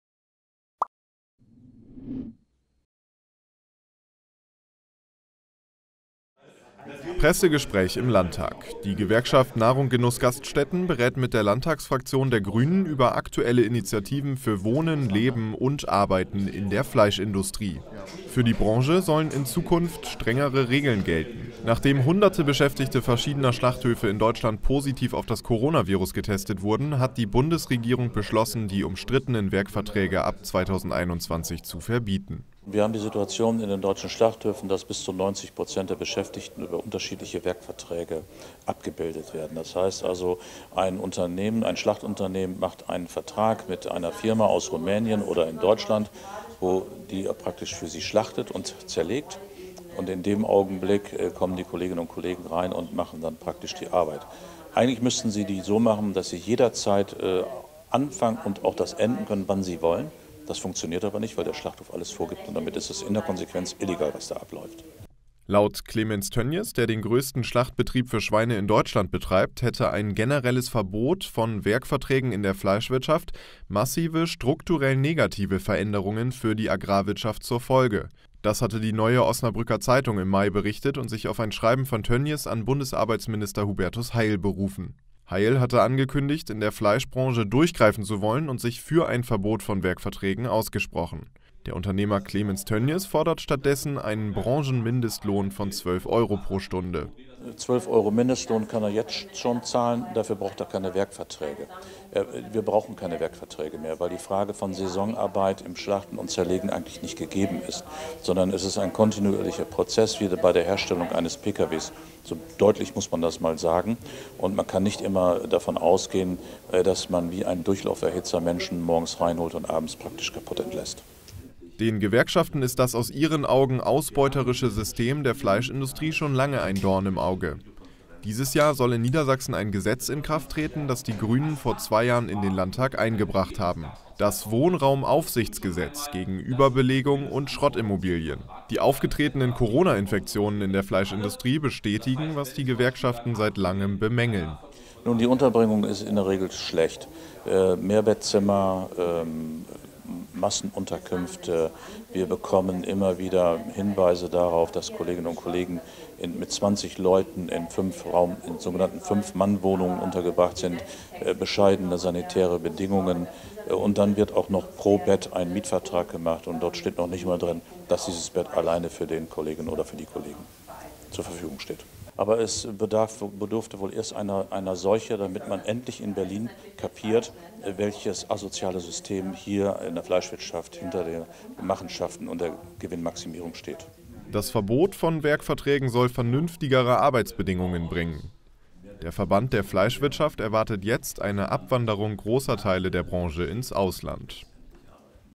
Schluss mit Werkverträgen – Fleischindustrie auf dem Prüfstand
Die rasante Ausbreitung des Coronavirus' unter Mitarbeitenden der Fleischindustrie hat wieder einmal verdeutlicht, in welchen Verhältnissen die häufig aus Osteuropa stammenden Personen untergebracht werden. Doch von Seiten der Politik scheint sich etwas zu tun: Mit Werkverträgen soll bald Schluss sein. Droht ein Abwandern der Branche ins Ausland? Bundesarbeitsminister Hubertus Heil und Unternehmer Clemens Tönnies haben unterschiedliche Auffassungen in Sachen Arbeitsrecht.

 Redaktion: Joscha Weidenfeld und Niklas Kuschkowitz
 Kamera: Lukas Liedtke
 Schnitt: Niklas Kuschkowitz